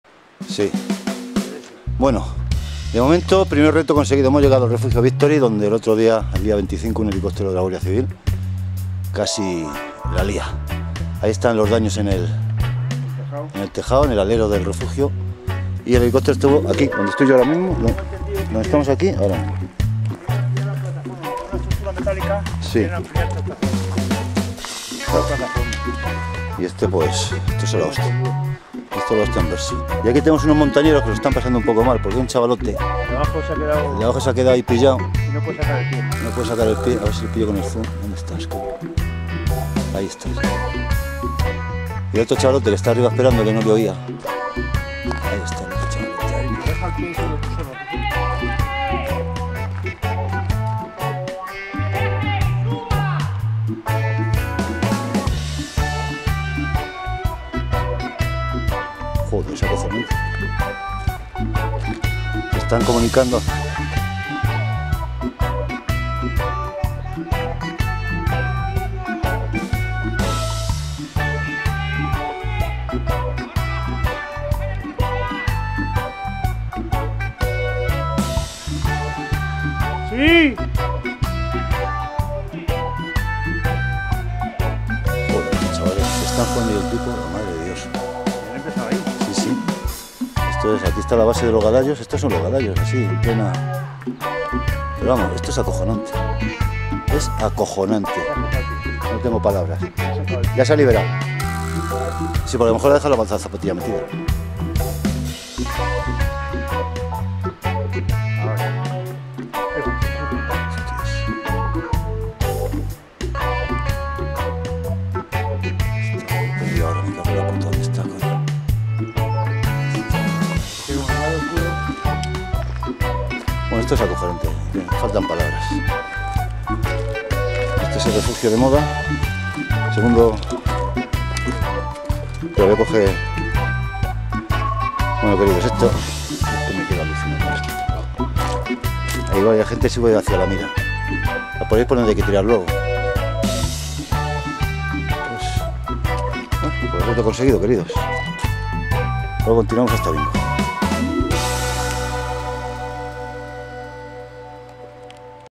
Los Galayos. Refugio Victory.
Video grabado el 11/05/2013 desde el refugio Victory a 1995 mts. Se puede apreciar en él a dos montañeros escalando el mítico Torreón de Los Galayos cuando sufren un pequeño percance del que afortunadamente salen airosos. Mis respetos hacia esta gente que son capaces de jugarse la vida por unas vistas únicas.